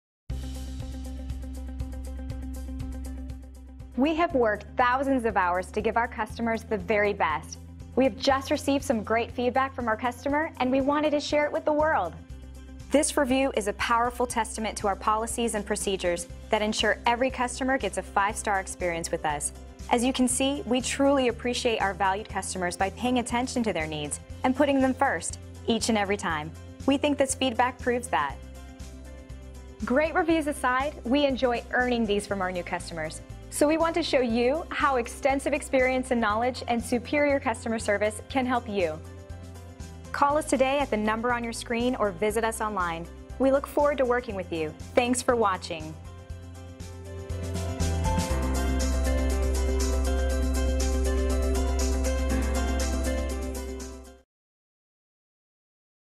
Affordable Carpet & Flooring Manassas Incredible Five Star Review by MARCIE KOHL
Excellent Rating

The flooring looks fabulous.  Dave was great to work with as well as his crew.  The job was not complete until we were happy.  That's great customer service!

Affordable Carpet & Flooring
8970 Mike Garcia Dr
Manassas VA
20109